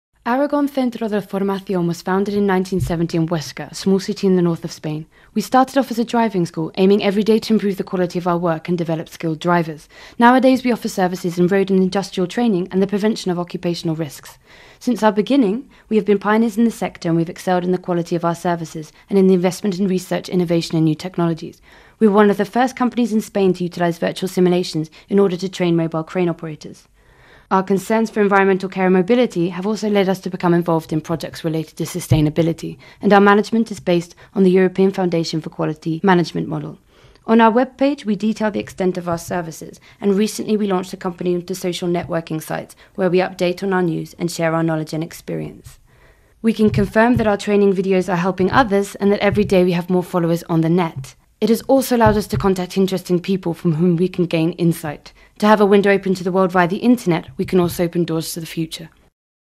Aragón Centro de Formación (English version)
Aragón Centro de Formación es una empresa fundada en 1970 en Huesca, una pequeña ciudad situada en el norte de España. Comenzamos como una autoescuela, con ganas de realizar nuestro trabajo cada día mejor y formar buenos conductores. En la actualidad, ofrecemos servicios de formación vial, formación industrial y prevención de riesgos laborales. Desde nuestros inicios hemos sido pioneros en el sector y hemos destacado por la calidad de nuestros servicios, por nuestra inversión en investigación, innovación y nuevas tecnologías. Por ejemplo, hemos sido una de las primeras empresas en España en utilizar simuladores virtuales para la formación de operadores de grúa móvil. Nuestra preocupación por el cuidado del medio ambiente y la movilidad nos ha llevado también a implicarnos en proyectos relacionados con la sostenibilidad. Y nuestra gestión se apoya en el modelo EFQM (European Foundation for Quality Management), teniendo como objetivo principal la satisfacción de todos nuestros grupos de interés.

Allí comentamos nuestras novedades e intentamos compartir nuestros conocimientos y experiencias. Es gratificante ver el efecto global: agradecen la difusión de nuestros vídeos, cada día contamos con más seguidores y hemos contactado con gente que también nos aporta nuevos conocimientos. Una ventana abierta al mundo a través de internet nos puede abrir también puertas al futuro.

(English version) Aragon Centro de formacion is a business founded in 1970 in Huesca, a small city in the north of Spain. We started off as a driving school, every day aiming to improve the quality of our work and develop skilled drivers. Nowadays, we offer services in road training, industrial training and the prevention of occupational risks.

Since our beginning, we have been pioneers in the sector and we have excelled in the quality of our services and in the investment in research, innovation and new technologies. For example, we were one of the first companies in Spain to utilize virtual simulations in order to train mobile crane operators.  Our concern about the care of the environment and mobility, has also led us to become involved in projects related to sustainability. And our management is based on the European model, EFQM (European Foundation for Quality Management) with the main objective being the satisfaction of all out stakeholders.

From our webpage, and our blog, we detail the extent of our services and only a few months ago, we launched the company onto social networking sites including Facebook, Twitter, LinkedIn and Youtube. From these locations we describe our latest innovations and we try to share with you our knowledge and experience. It´s rewarding to see its overall effect; thanks to the widespread diffusion of our videos, every day we have more followers on the net and have obtained contact with people who also provide us with new insights. With a window open to the world via the internet, we can also open doors to the future.

VOZ VÍDEO: Mo Pearce